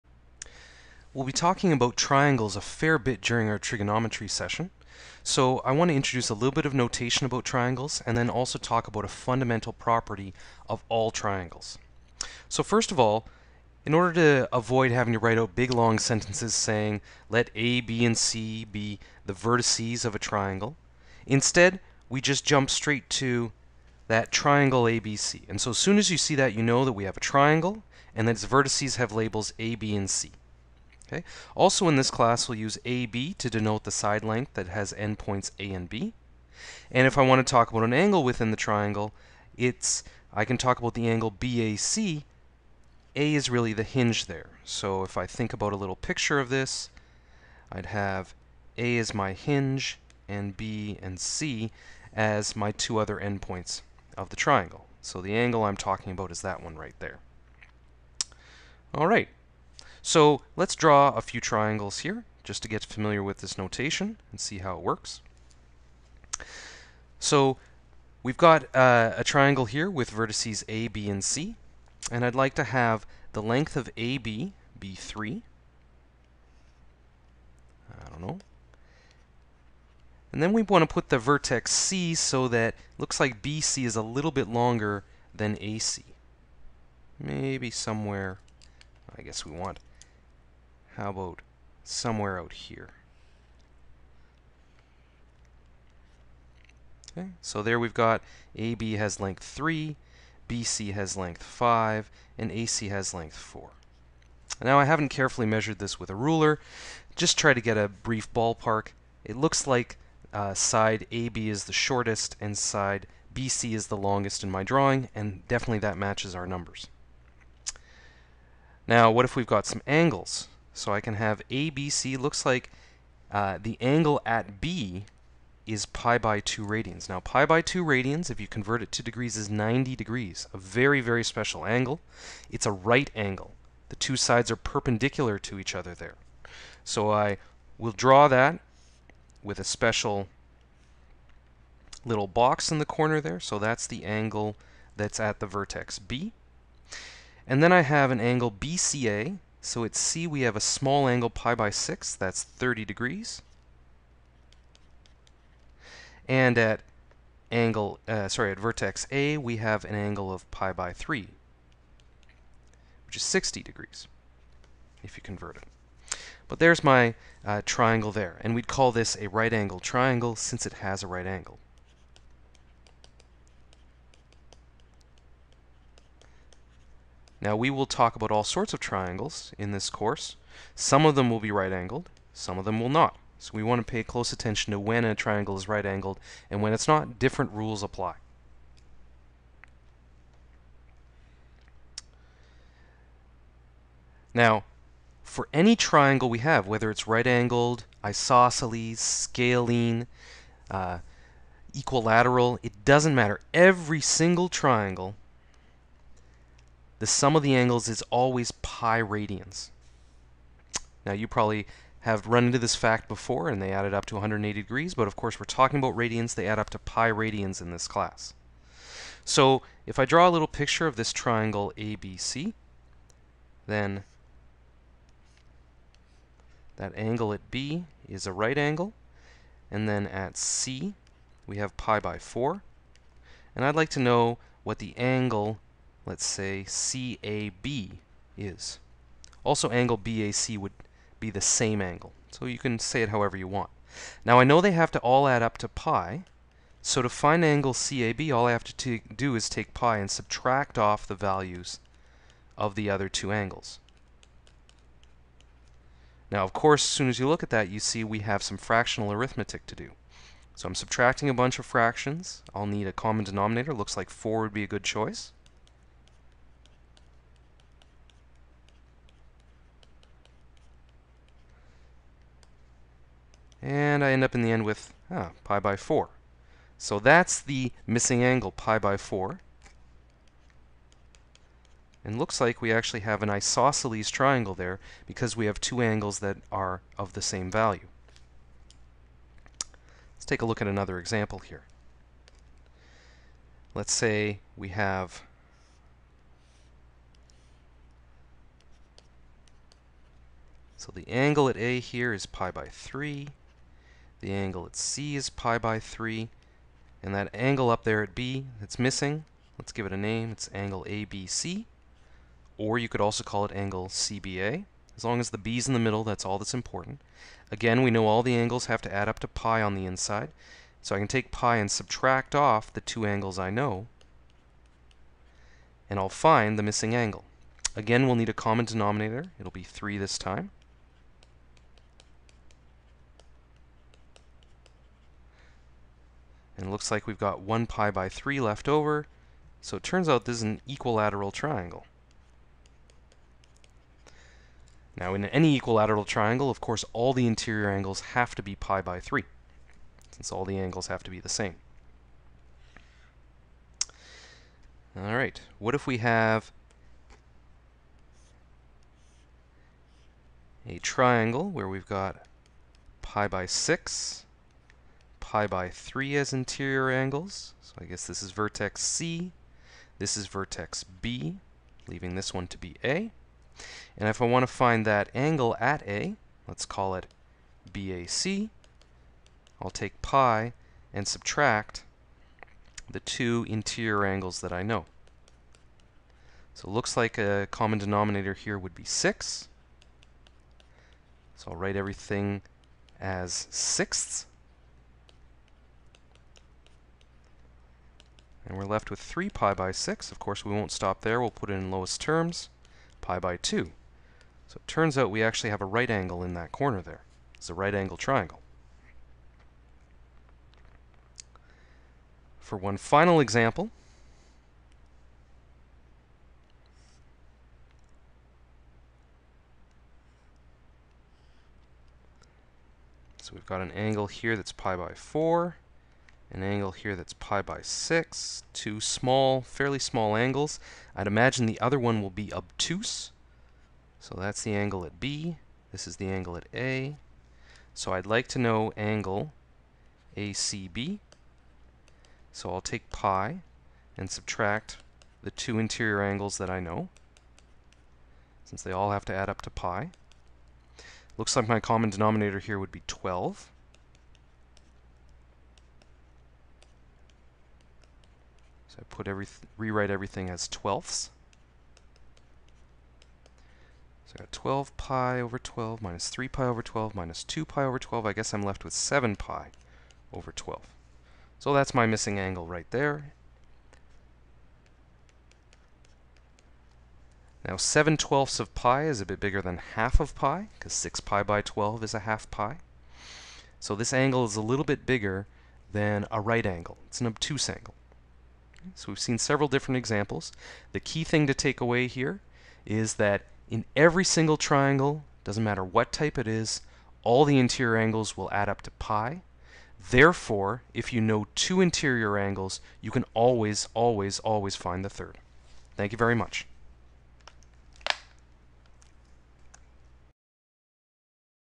Triangle Notation
We review some notation to help us efficiently discuss triangles, their side lengths, and their angles.  We find the missing interior angle of any triangle given the values of the other two interior angles.